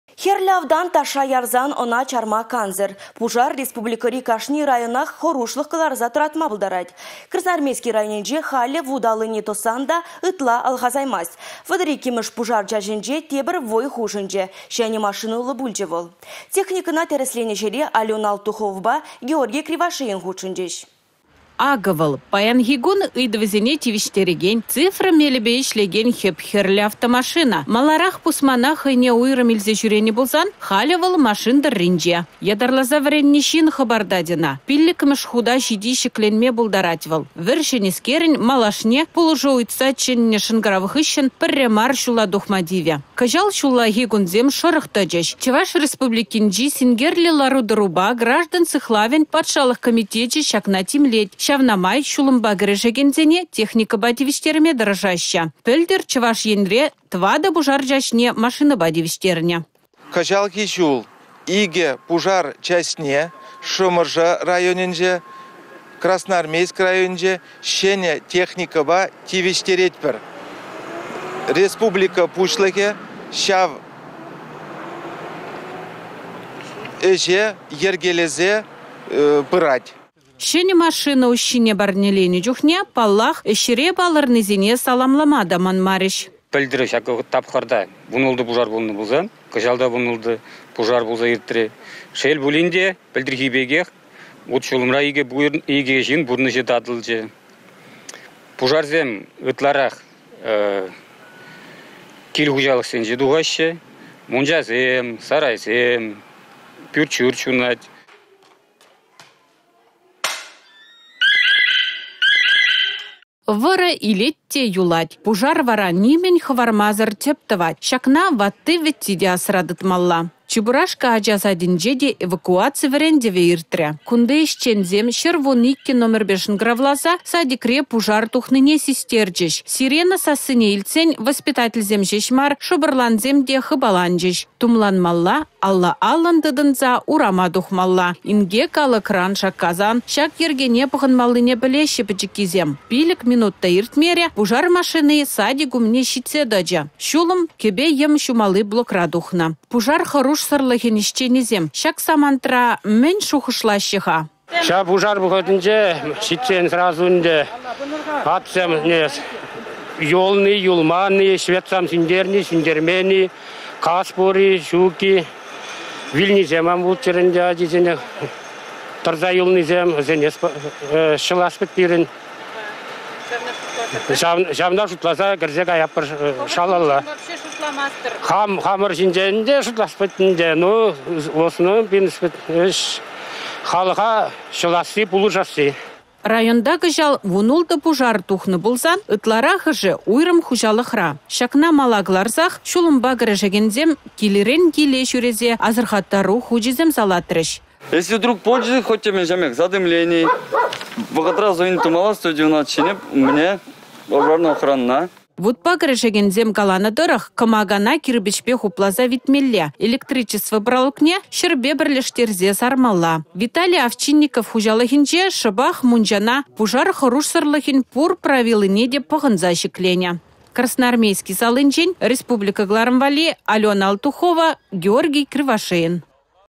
Пушар техникине тĕрĕслерĕç
Хĕрлĕ автан «ташша ярсан» ăна чарма кансĕр. Пушар республикăри кашни районах хăрушлăх кăларса тăратма пултарать. Красноармейски районĕнче халĕ вут аллине тăссан та ытла алхасаймасть. 32-мĕш пушар чаçĕнче тепĕр вăй хушăнчĕ - çĕнĕ машинăллă пулчĕ вăл. Техникăна тĕрĕсленĕ çĕре Алена Алтуховăпа Георгий Кривошеин хутшăнчĕç.